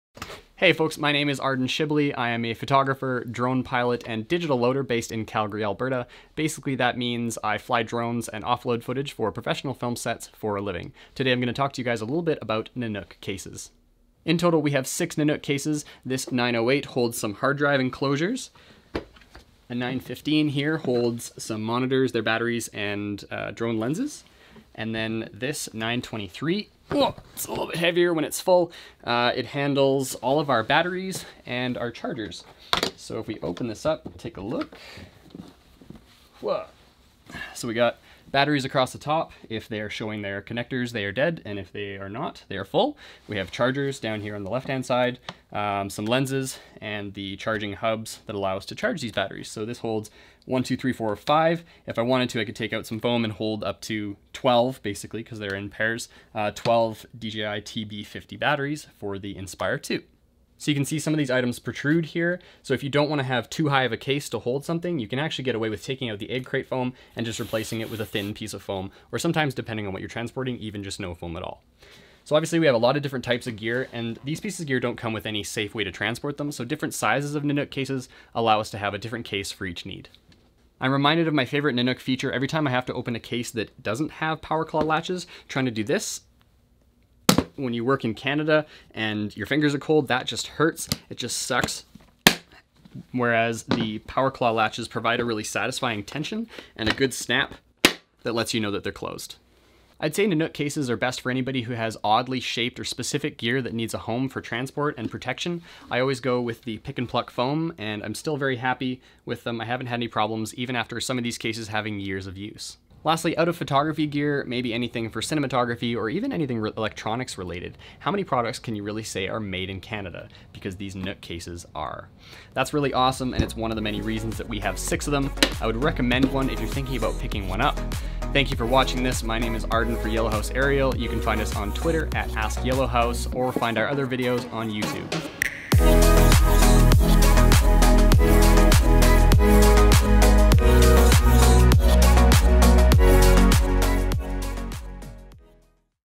Nanuk Case Review by Arden Shibley of Yellowhouse Aerial
When working for his clients in high altitude or in the middle of agricultural fields, Arden Shibley from Yellowhouse Aerial uses the highest quality (and most expensive) gear.

And to protect his precious gear, Arden uses Nanuk Cases.

In this video, he talks about what he likes in Nanuk over Pelican cases and why he thinks they are worth every penny.